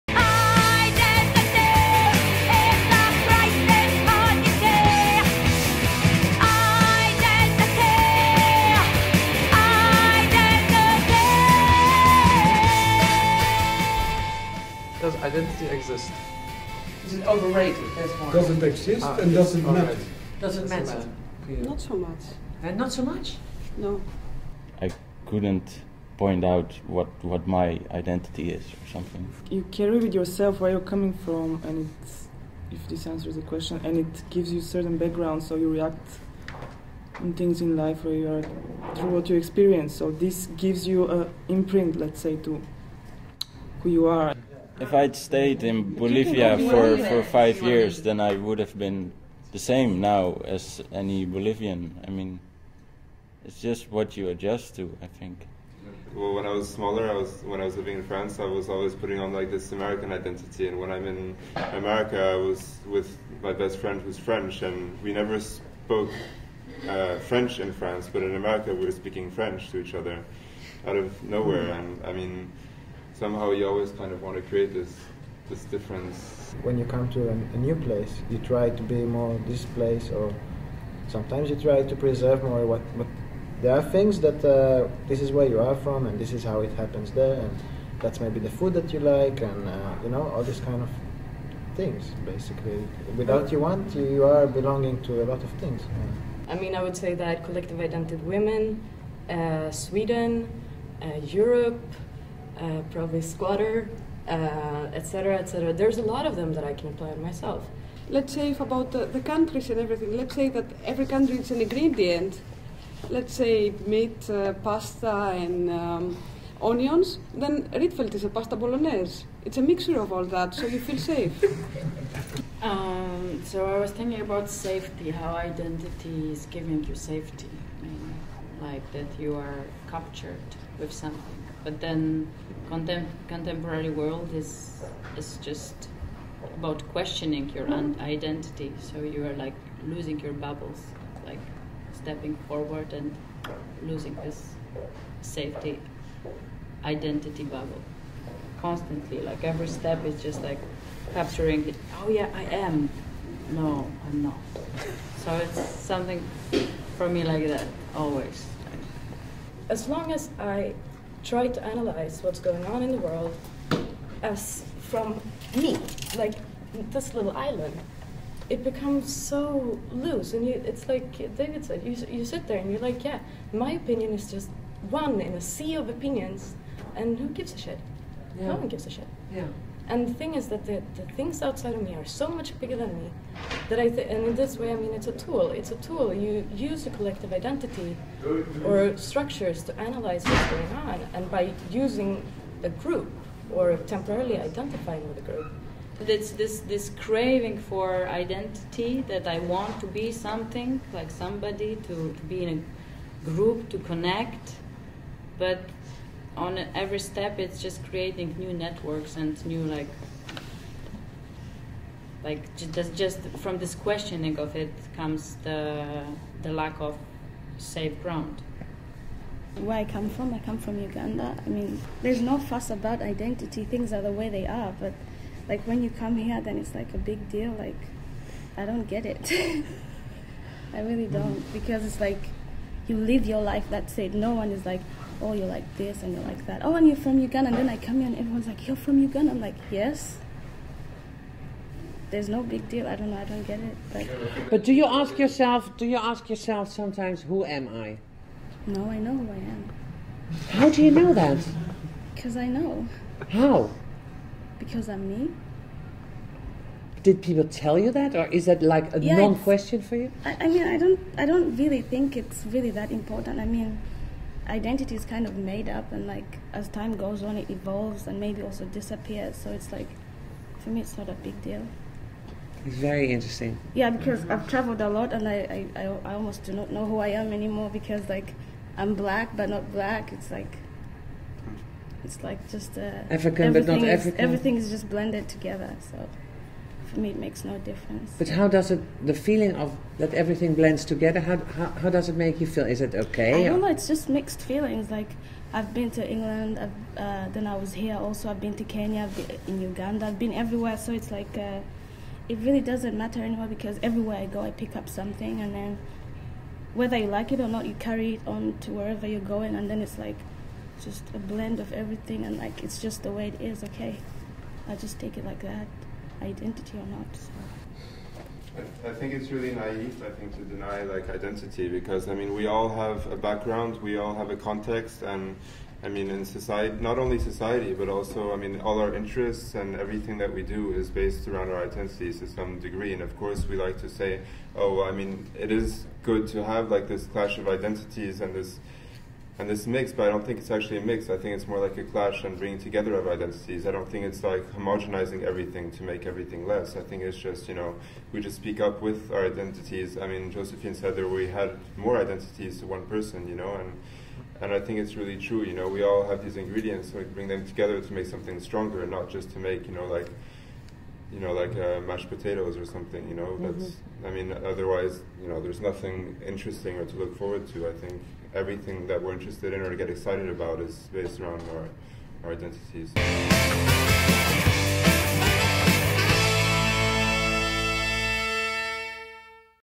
Discussing identity with students from the Rietveld Academy
This footage is also from the debate with the students from the Rietveld Academy (a well known Dutch art school.)

This discussion focuses on the question of 'identity'. Does it exist and is it important to have one?